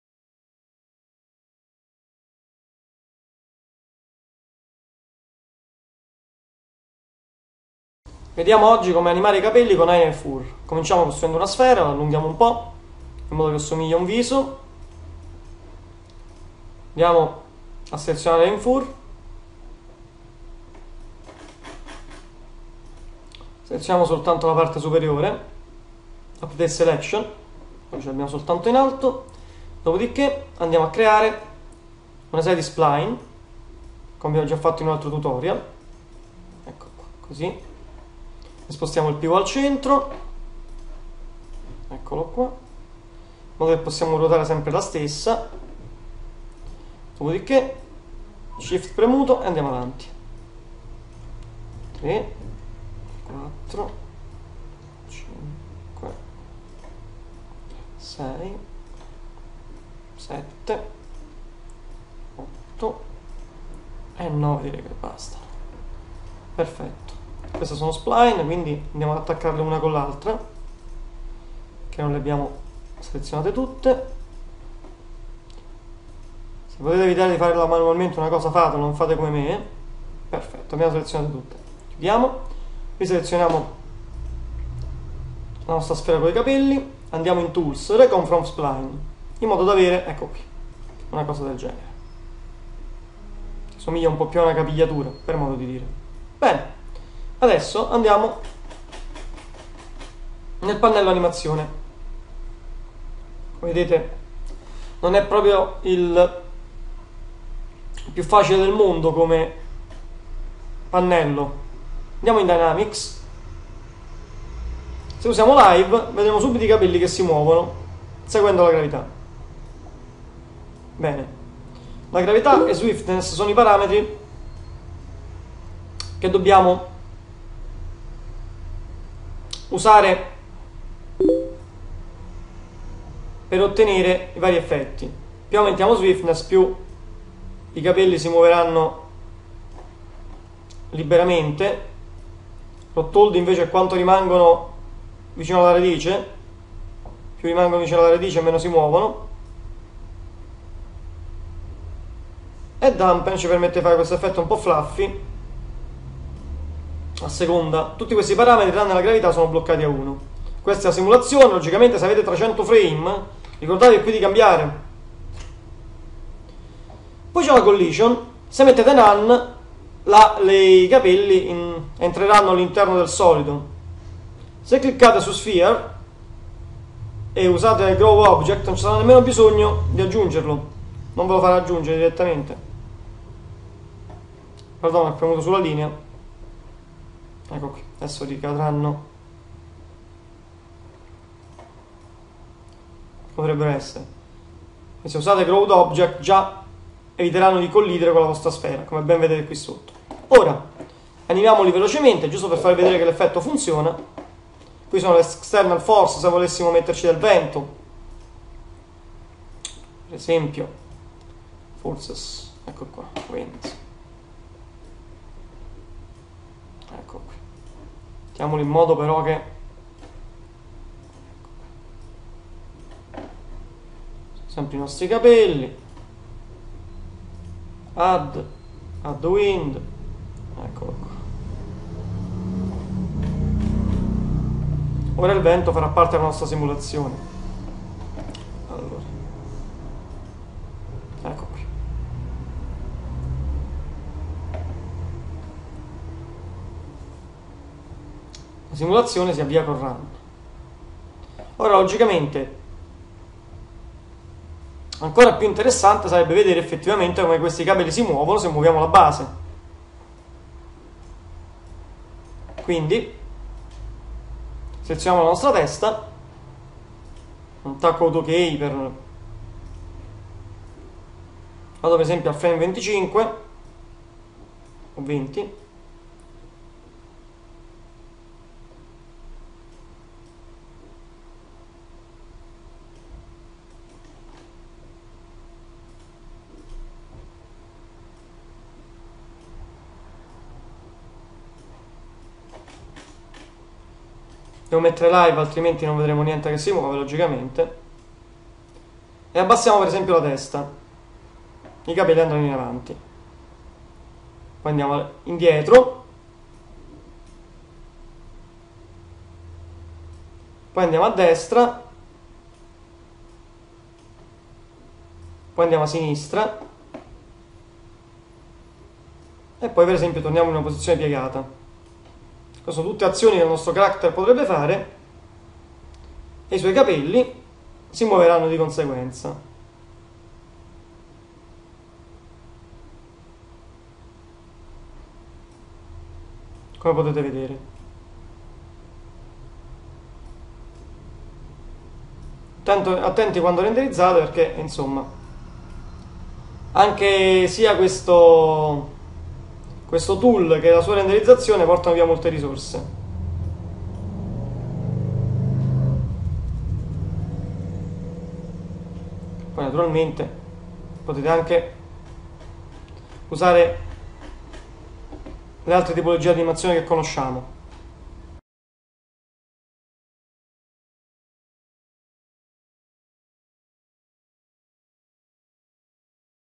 Videotutorial Autodesk 3dsmax - #64 Hair and Fur Basic Animation
Veloce tutorial sull'animazione dei capelli creati con Hair and Fur. Questo comando di 3dsmax è molto versatile ma anche molto complesso, ci permette, però, di animare anche l'erba. Normalmente i capelli non vengono animati, nemmeno nei videogiochi, tuttavia è una tecnica che è bene conoscere. Blog

Quick tutorial on animating hair created with Hair and Fur. This 3dsmax command is very versatile but also very complex, allows us, however, of animating grass. Normally the hair are not animated, even in video games, however, is a technique that is better to known. Blog

髪の毛や毛皮で作成された髪をアニメーションに簡単なチュートリアル。この3DSMAXコマンドは、草をアニメーションに、しかし、非常に汎用性だけでなく、非常に複雑で、私たちを可能にする。通常の髪をしても、ビデオゲームでは、しかし、知られた方が良い手法である、アニメーションされていません。